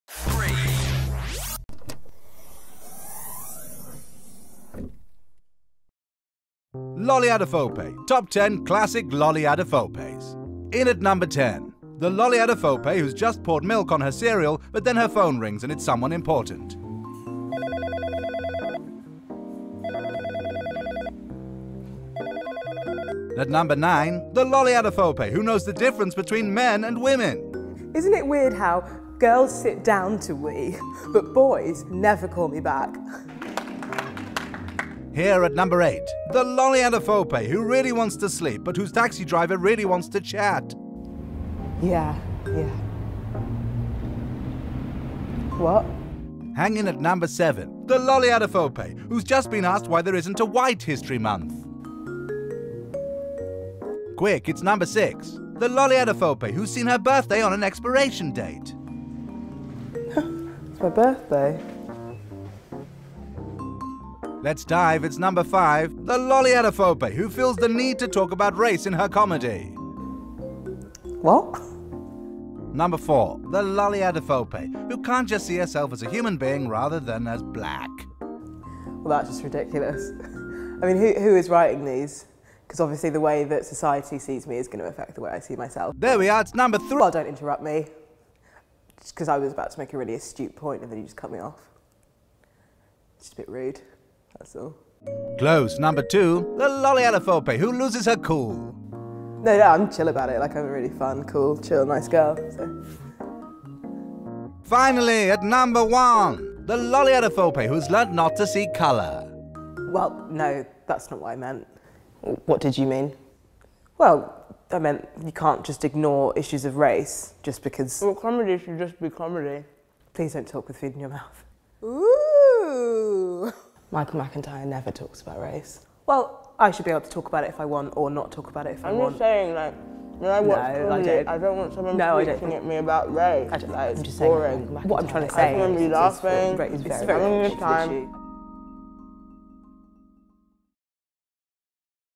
Lolly Adefope's Top Ten Classic Lolly Adefopes
Lolly Adefope lists her top ten Classic Lolly Adefope moments.